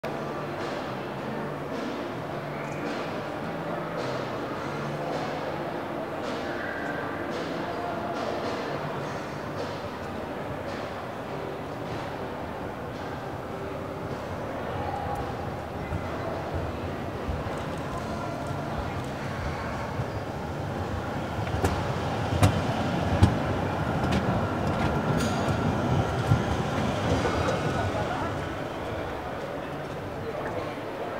VhAG BOGESTRA e.V. - GT6 Wagen 40 / Sonderfahrt
Tag der offenen Tür bei der Ruhrbahn in Mülheim an der Ruhr

Links daneben stand ein M6C-NF abgestellt.